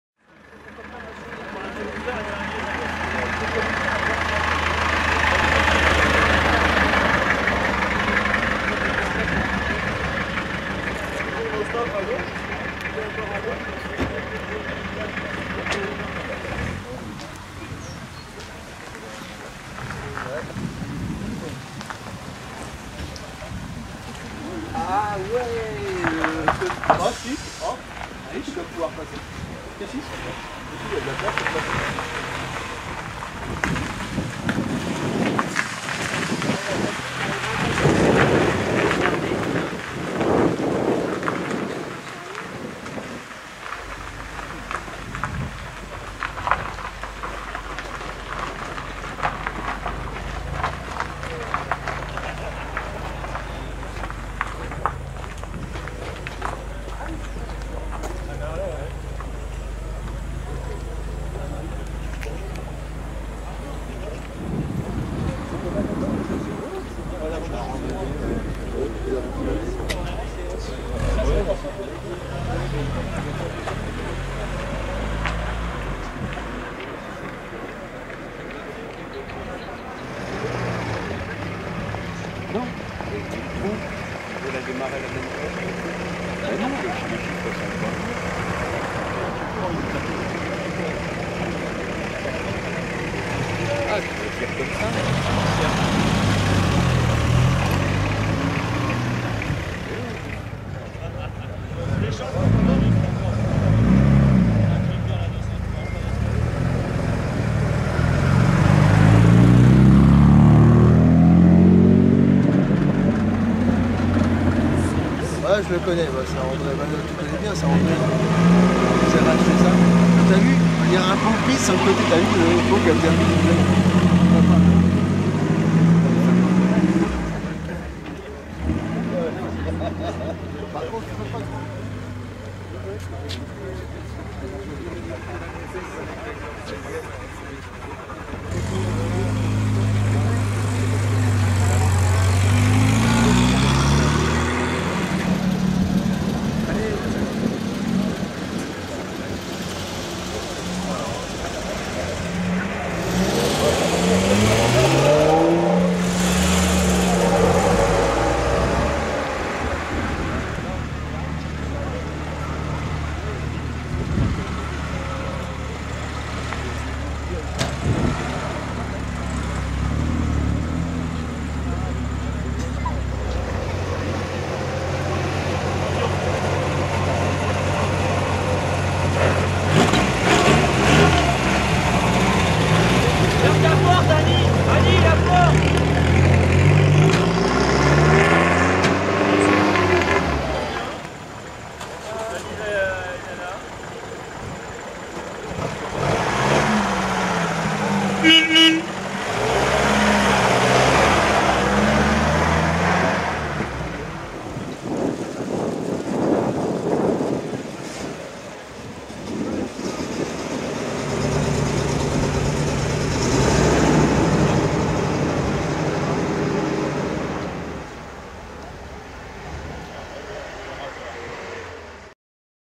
Fans d'Anciennes à Epinay-sur-Orge - 13 octobre 2013 - 3ème partie
Rendez-Vous mensuel de voitures anciennes organisé par l'association "Fans d'Anciennes" ) se tenant tous les 2ème dimanche du mois autour de la mairie d'Epinay-Sur-Orge (91).